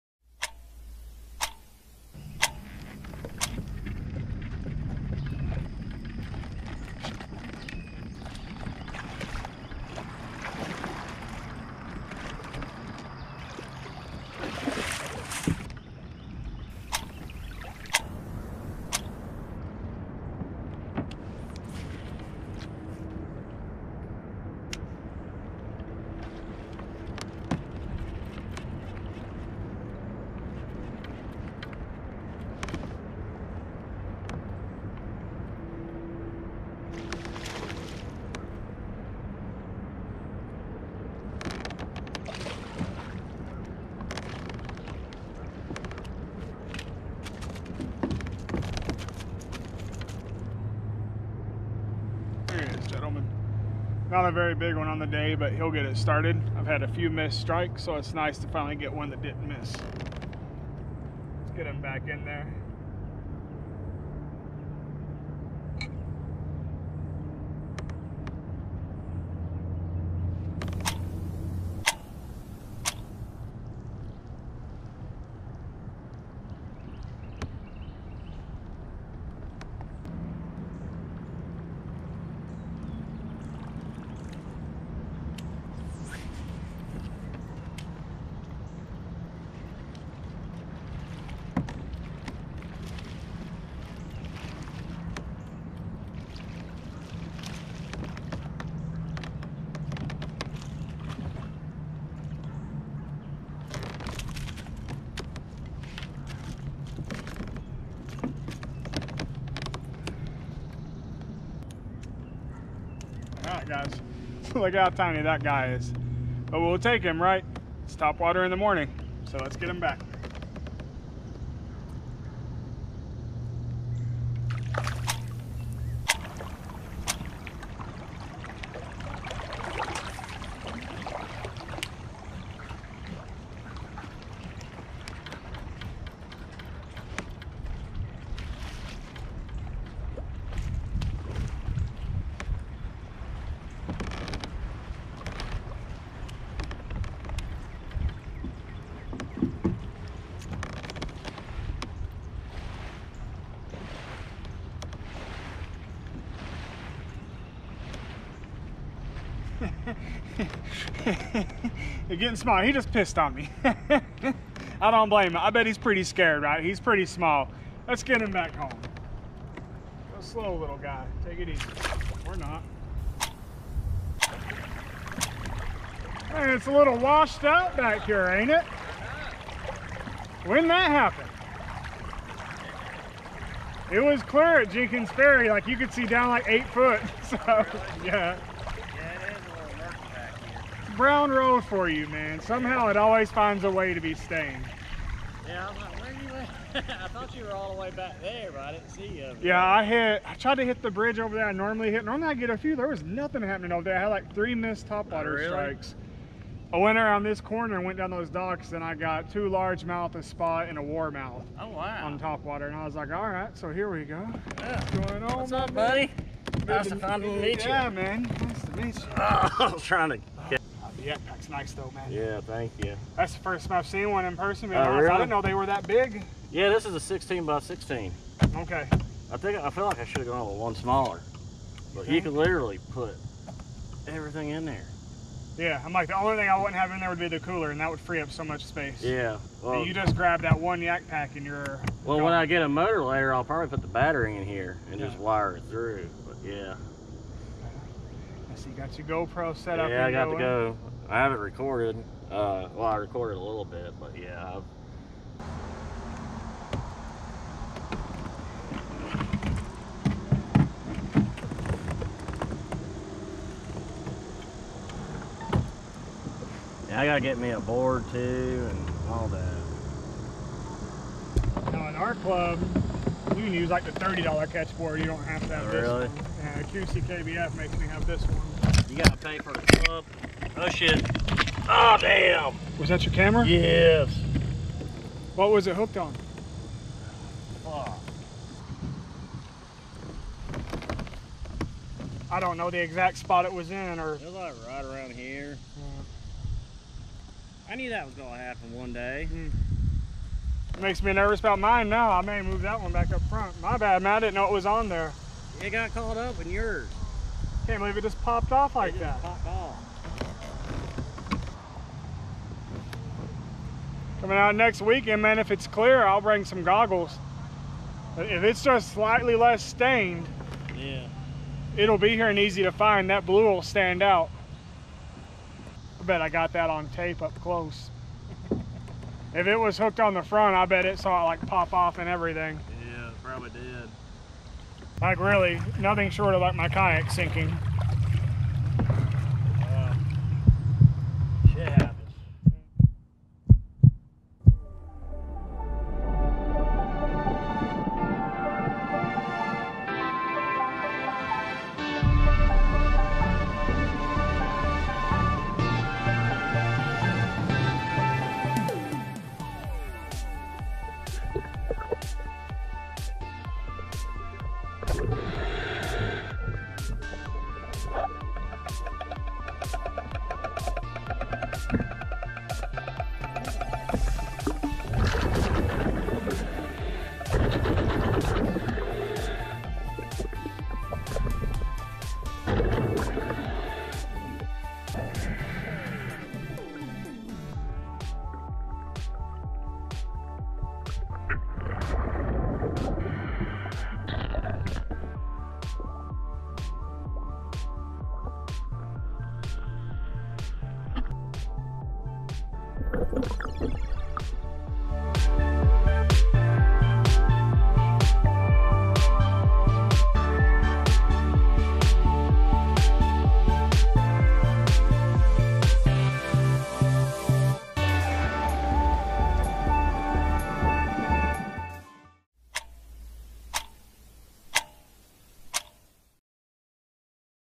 Lake Hartwell Summer Kayak Bass Fishing - Go Pro Lost and Found - Brown Road Boat Ramp
In today's video, I visit Brown Road Boat Ramp on Lake Hartwell for some September kayak bass fishing. I was fishing with my friend Eric Sloan when at the end of the day, disaster happened. Go...Pro...Down!

The video shared at the end belongs to @effinericsloan and you should go check his YouTube and Instagram...

Eric was able to get in touch with Lake Hartwell Divers and they were able to recover his GoPro for him. They take donations, so if you need them, just reach out and offer them something. I plan to get some gear off the Amazon year, maybe this group can tell me how to be safe down there.

0:00 Intro
0:18 Fish
1:10 Fish
2:05 Fish
2:53 Meet Eric
4:13 GoPro Down!
5:42 Lake Hartwell Divers